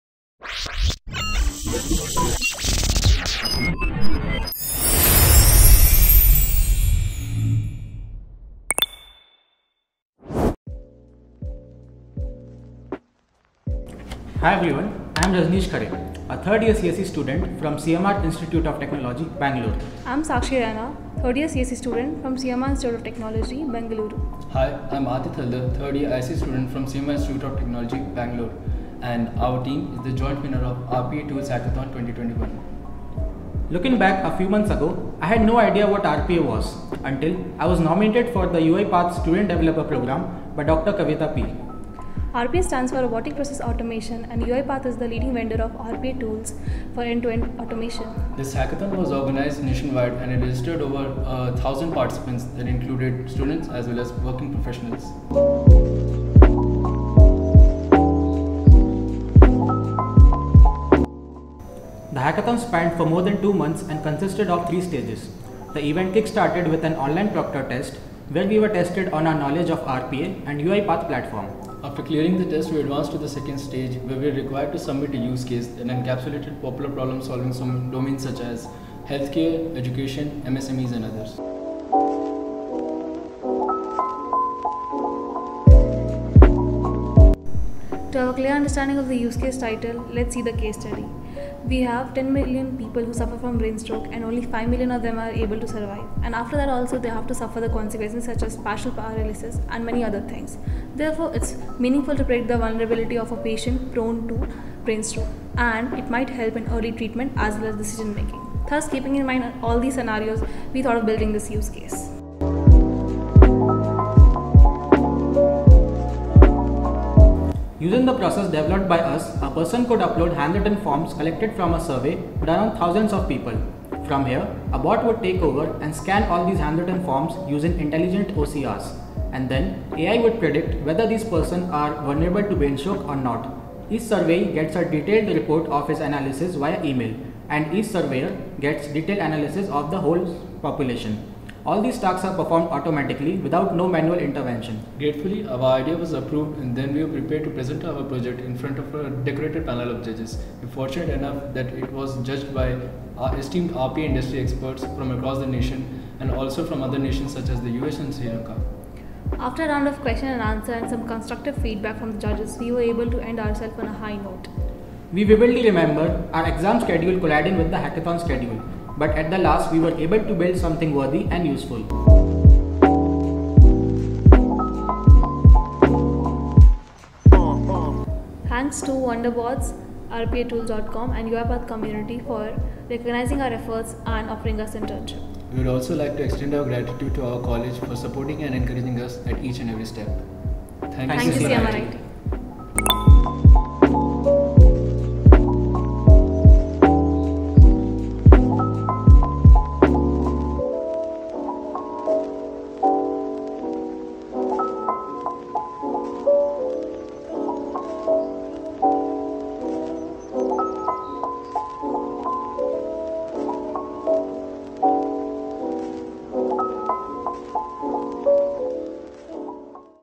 UIPath | RPATools Hackathon 2021 | Joint Winners | CMRIT | Bangalore
Hearty Congratulations to Rajneesh Khare (CSE), Sakshi Raina (CSE) and Aditya Haldar (ISE) on being the Joint Winners of the RPATools Hackathon 2021. The winners' cash price was Rs. 49,000/-. They presented the use case Brain Stroke Prediction Application using UIPath Platform.

You have made all CMRITians proud of you!!! Keep it up!